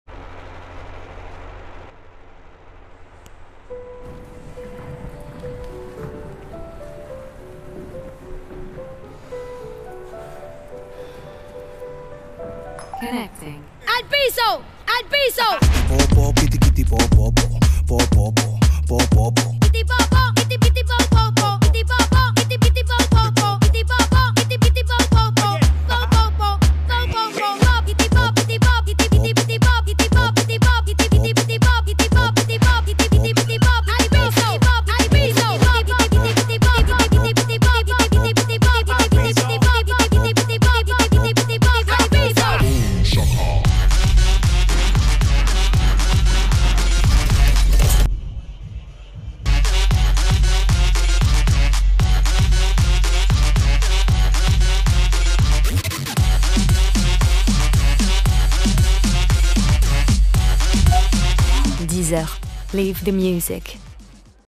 Deezer - Live The Music
Agency  : BETC Paris
Campaign : Live The Music
Advertiser : Deezer
Brand  : Deezer
Length  : 1 Minute, 4 Seconds
Posted   : Nov 18, 2023

In this Movie Ad we’ll walk you through:
 In a film directed by Nogari, amateur ballet dancers experience a lively shift in their class vibe due to an unexpected change in music.

TIMESTAMPS :
00:00 Deezer - Live The Music
Plot :
"Live The Music" follows a group of amateur ballet dancers in their weekly class. A sudden change in music transforms the atmosphere, prompting the dancers to break free from the class's constraints and engage in a spontaneous and liberating dance. The lively choreography adapts to subsequent changes in music genres, creating a joyful and unconventional dance experience. The twist comes when it's revealed that a passerby accidentally connected her Deezer app to the ballet studio, causing the delightful chaos.